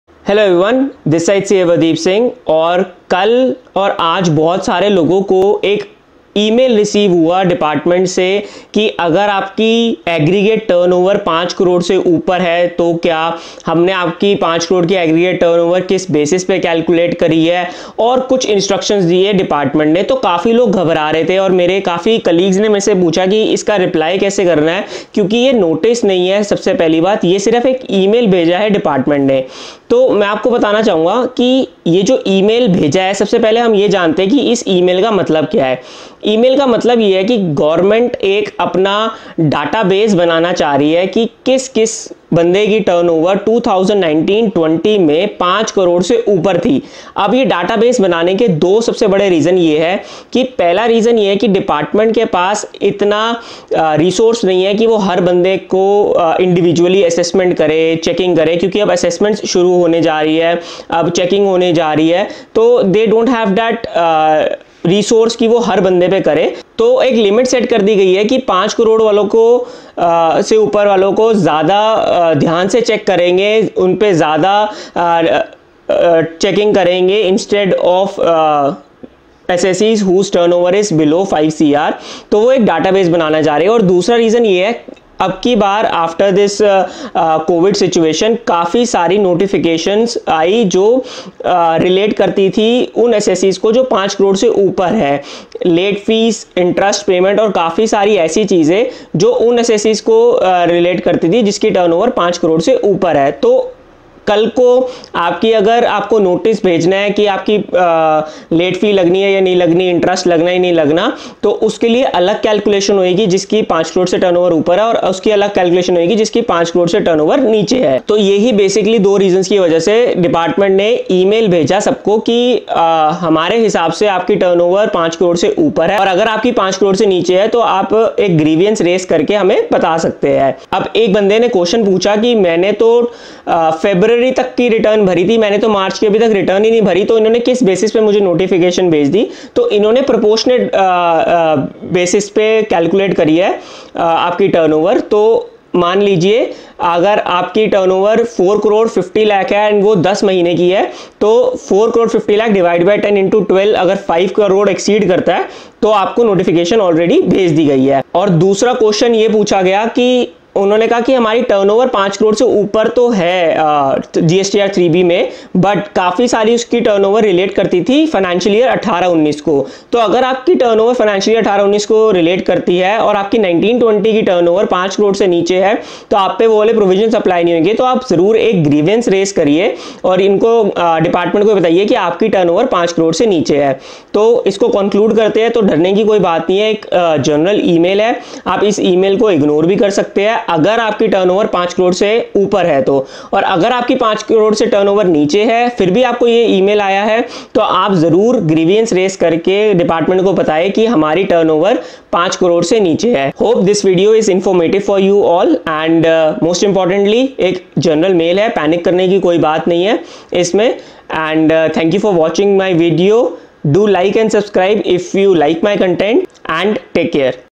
Department se aaya Email regarding turnover issue || How to reply ??
Today, many assessee's received an email from department regarding the clarification on turnover.

Here is a detailed explanation on what the email trying to communicate and how we should deal with the same.